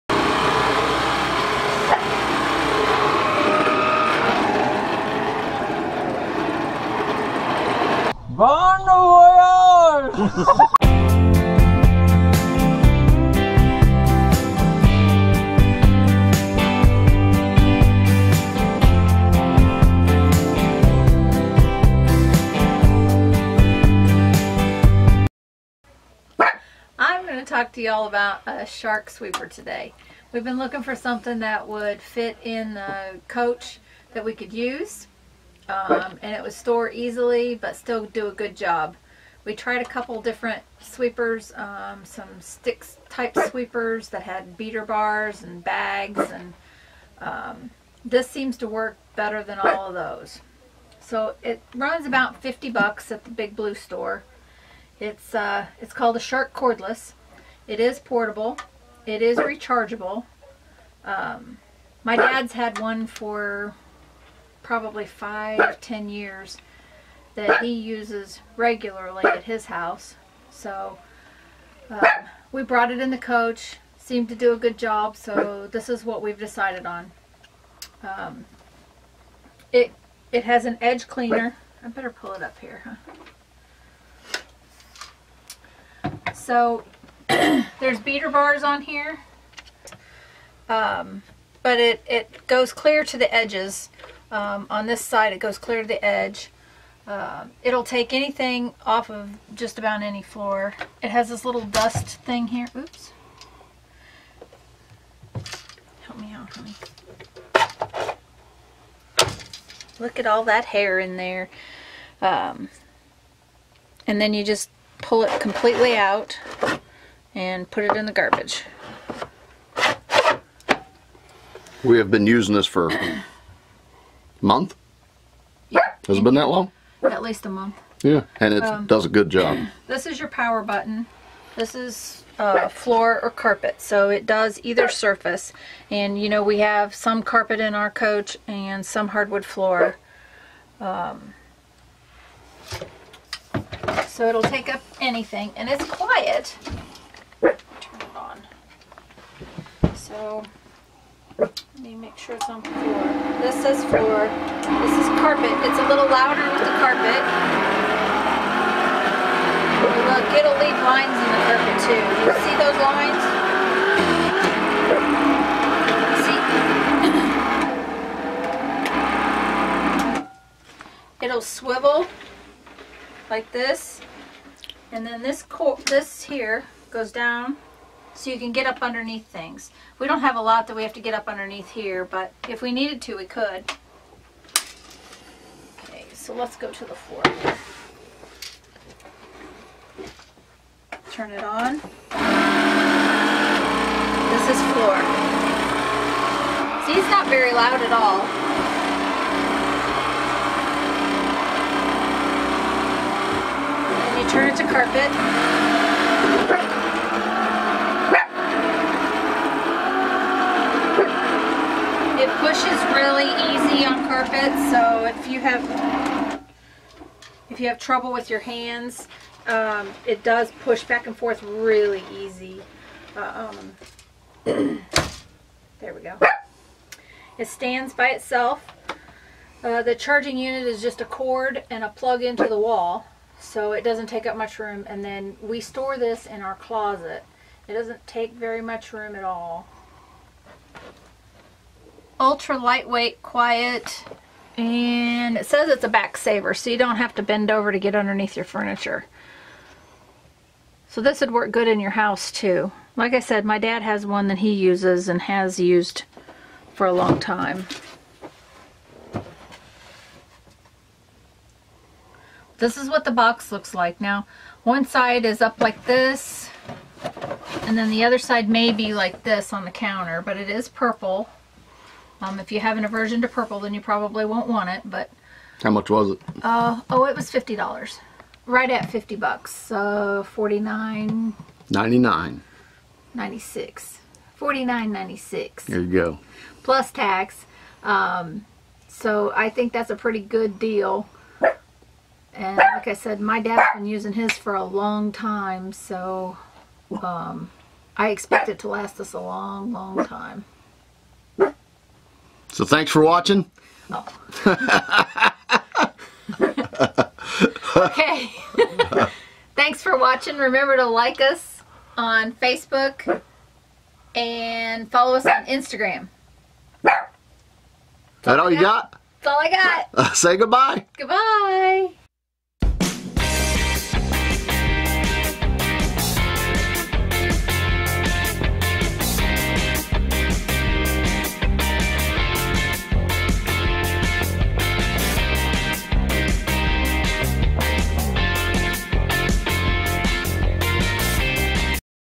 Shark Cordless Sweeper
Shark Cordless  Sweeper, We found what we think is a great RV Sweeper. Small,  Rechargeable and Light Weight.
We Bought it at the Big Blue Store $49.88
So far it has done a Great Job